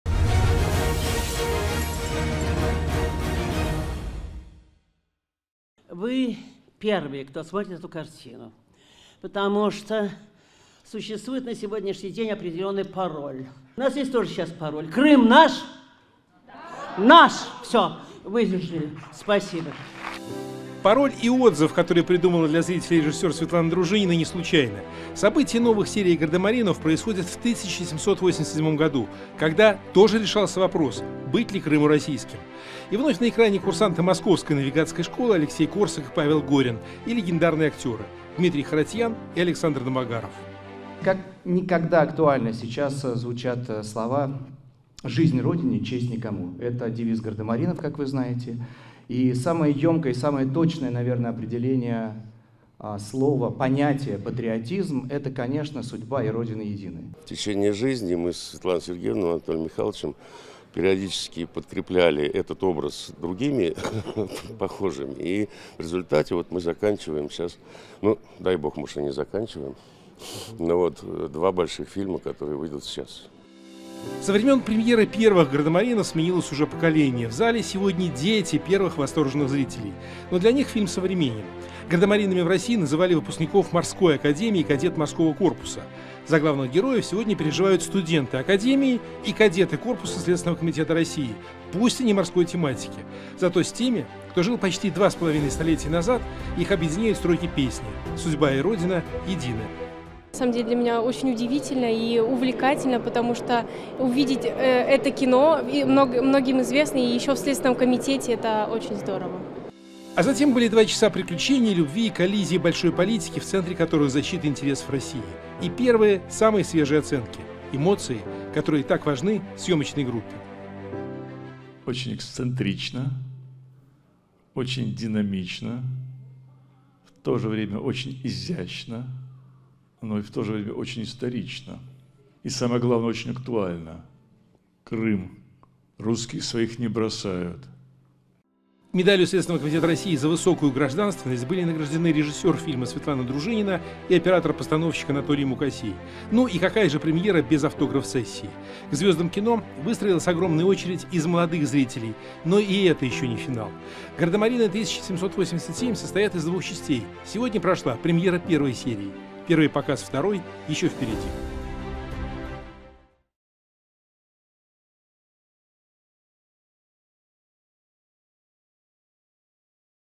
В Следственном комитете России состоялся предпремьерный показ фильма «Гардемарины 1787. Мир»
В киноконцертном зале Следственного комитета Российской Федерации состоялся предпремьерный показ фильма «Гардемарины 1787. Мир». Картина является продолжением культового кинематографического цикла о гардемаринах.

Мероприятие  организовано Культурным центром СК России. В нем приняли участие Председатель Следственного комитета России Александр Иванович Бастрыкин, его заместители, руководители и сотрудники подразделений центрального аппарата, обучающиеся ведомственных образовательных учреждений, заслуженный юрист РФ член Общественного совета при Следственном комитете России Александр Григорьевич Звягинцев.

Среди почетных гостей - представители творческой группы: режиссер-постановщик народная артистка РФ Светлана Сергеевна Дружинина, оператор-постановщик  народный артист РФ Анатолий Михайлович Мукасей, народные артисты России Дмитрий Вадимович Харатьян и Александр Юрьевич Домогаров, народный артист РСФСР Всеволод Николаевич Шиловский, заслуженная артистка России Ольга Владимировна Машная, начинающие  актеры Андрей Лаптев и Ника Здорик.

В преддверии просмотра с приветственным словом к гостям мероприятия обратилась С.С. Дружинина, которая рассказала присутствующим об истории создания фильма. Также зрителей поприветствовали актеры и создатели фильма.

Сюжет новой киноленты развивается во времена Екатерины II. Упорная борьба за Крым порождает множество международных интриг вокруг российского престола. Европейские государства категорически не желают усиления России, начинаются шантаж и интриги. Как и прежде, императрица призывает своих верных гардемаринов раскрыть заговор.

В киноленте вновь звучат как никогда актуальные для россиян девизы «Жизнь Родине – Честь никому» и «Судьба и Родина – едины!».

Съемки картины проходили в Крыму, Переславле-Залесском, Санкт-Петербурге и его окрестностях.

После просмотра глава СК России высоко оценил киноповесть, назвав её «прекрасной,  актуальной и духоподъёмной работой». Он отметил ее особую важность в это непростое для Отчизны время. «Президентом России подписан Указ об утверждении Основ государственной политики по сохранению и укреплению традиционных российских духовно-нравственных ценностей. В фильме отражены принципы суверенности и независимости России.  У нас есть своя история и традиции. Мы помним подвиги прадедов и отцов, и эта связь поколений хорошо показана в фильме», - отметил глава СК России.

Председатель СК России поблагодарил творческую группу и артистов за инициативу, вручив ведомственные медали «За высокую гражданственность» и благодарности.

В завершение мероприятия состоялась автограф-сессия. В ближайшее время состоится предпремьерный показ следующей части киноэпопеи.